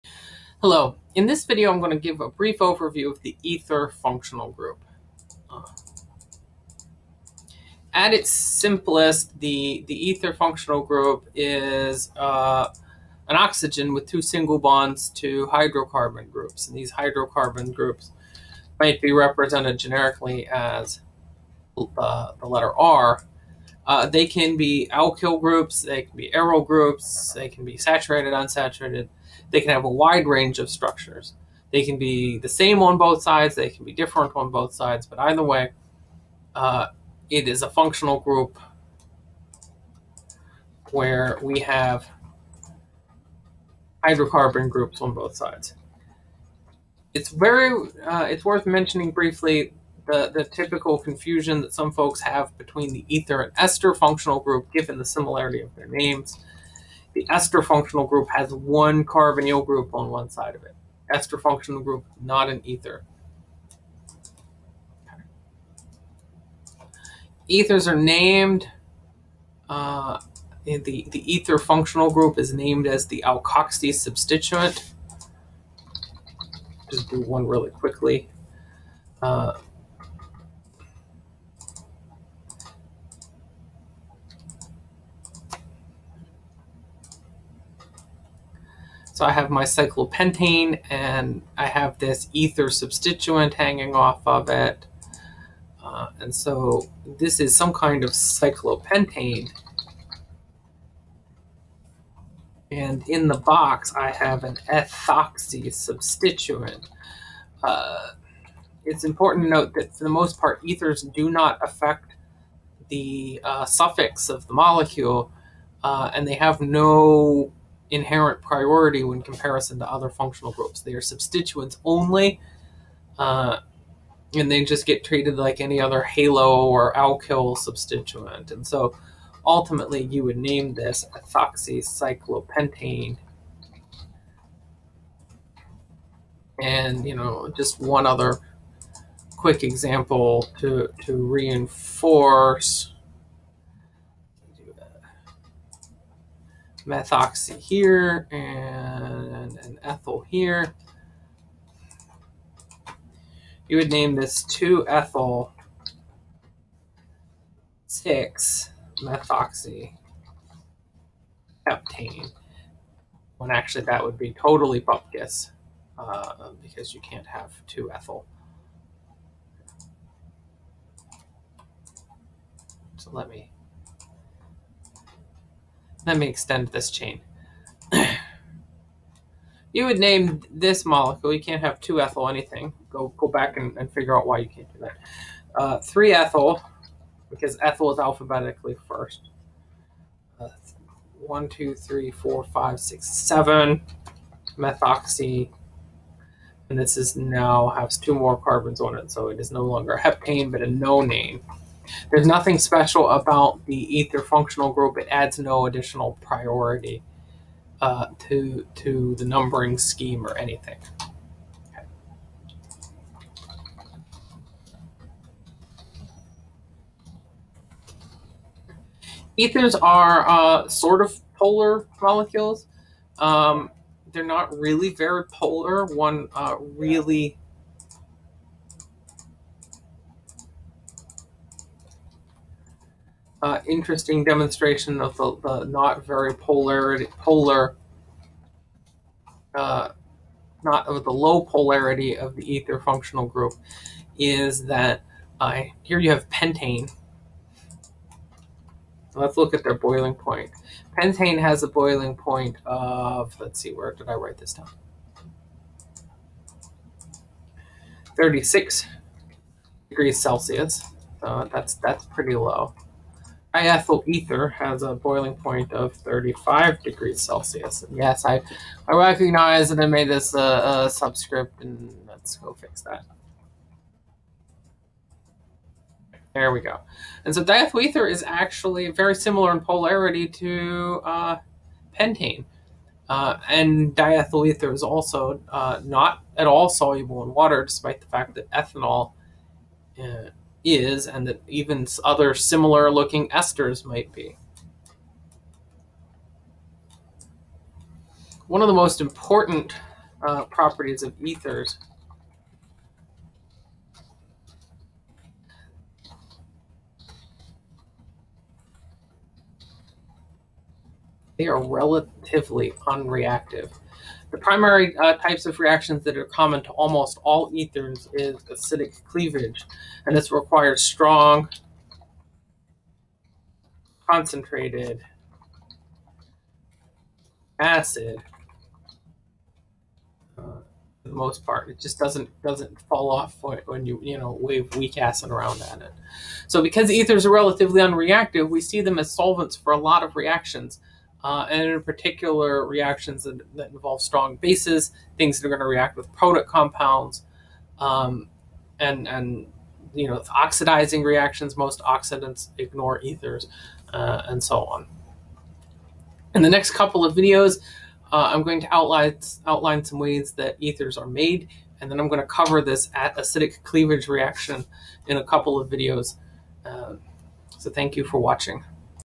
Introduction to Ethers
Dr. Norris describes the properties and nomenclature of ethers.